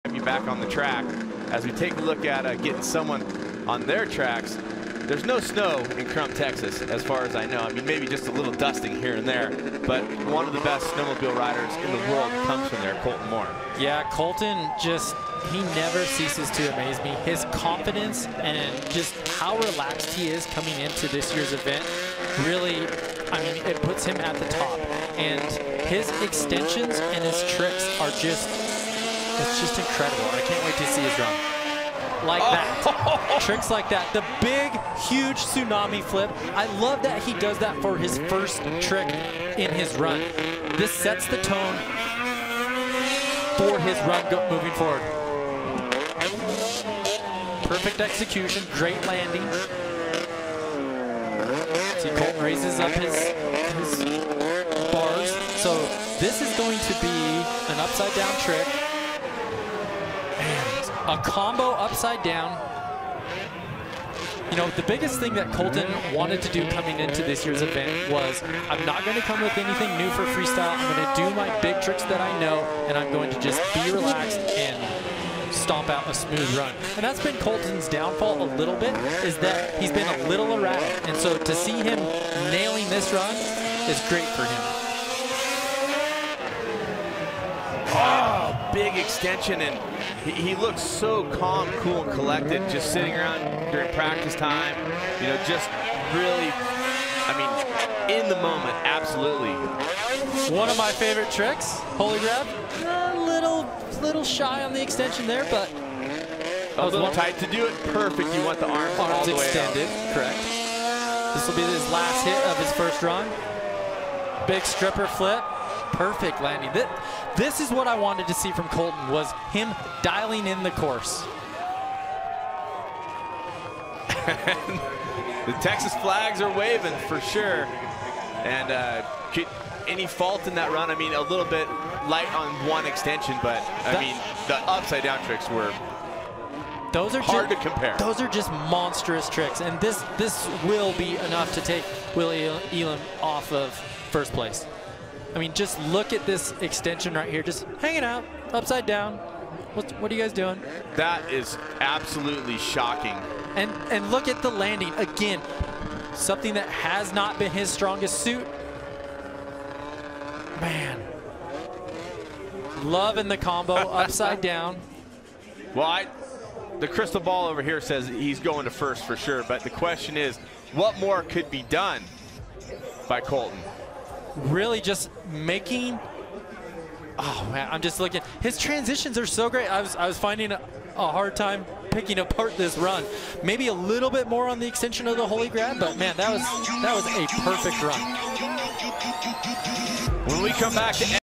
Colten Moore wins Snowmobile Freestyle silver
Colten Moore wins the silver medal in Snowmobile Freestyle, Saturday at X Games Aspen 2017.